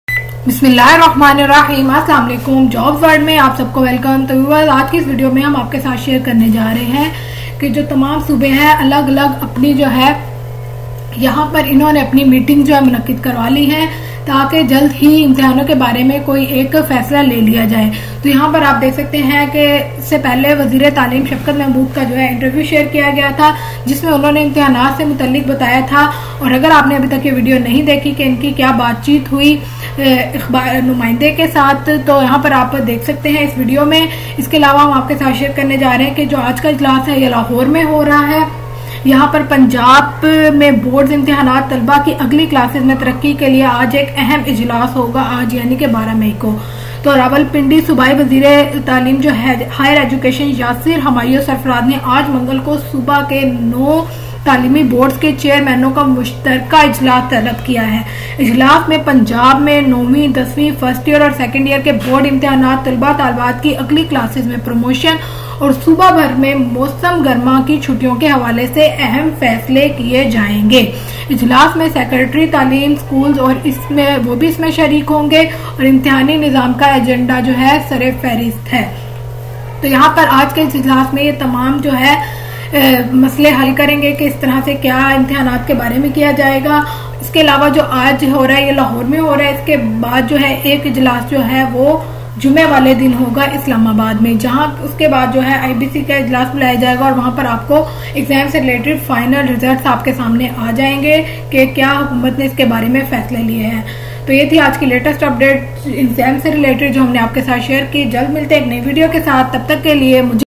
All Boards Meeting Held in Lahore 12 May | Latest Update about Board Exams | Good News for Students
In this video we share updates of Board Exams 2020. All provinces held Board Exams Meeting to take final decision about Board Exams 2020.
We flash the key points of meeting in this video. Also takes decision about school opening in July.